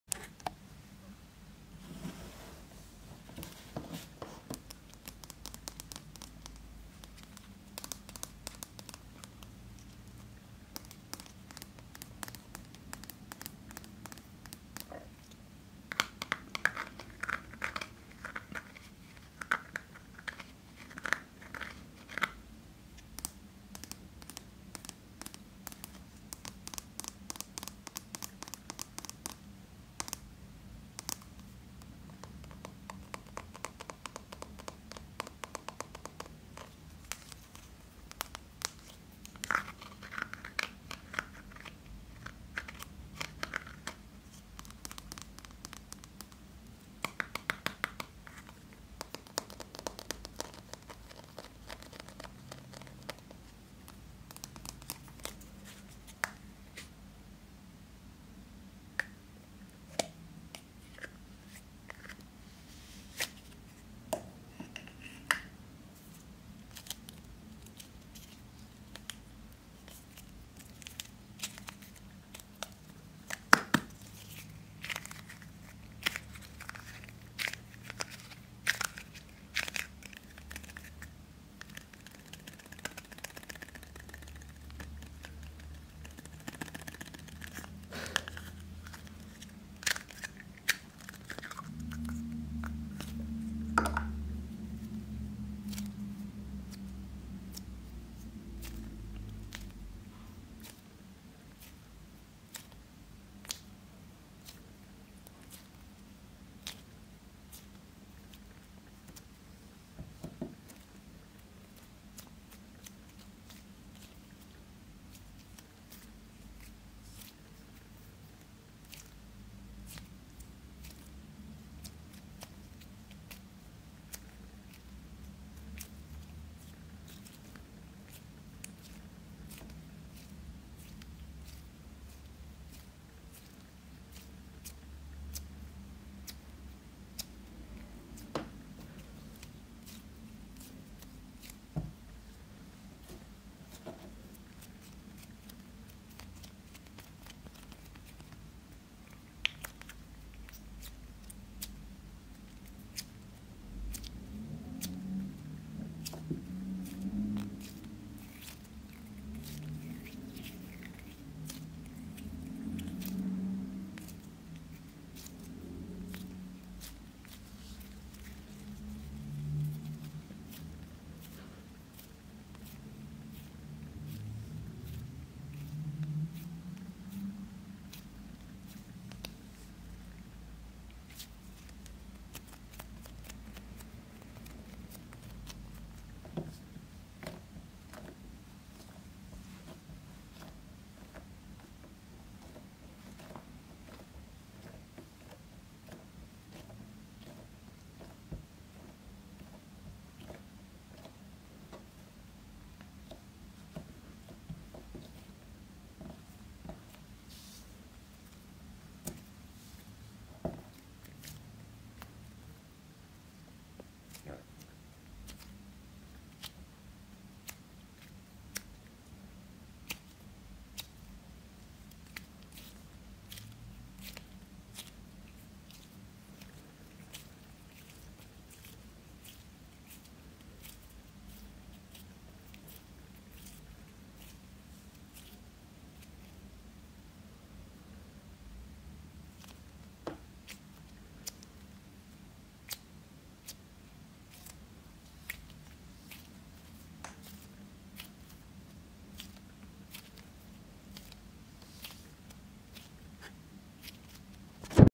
ASMR ~ Playing with Play-Doh *Making up for the failed video* (no talking)
In this video I simply squeeze and flatten Play-Doh. This video was made to make up for the time lost in the 'Paint Peeling' video.

My goal is to provide sounds that will bring you relaxation and/or give you tingles.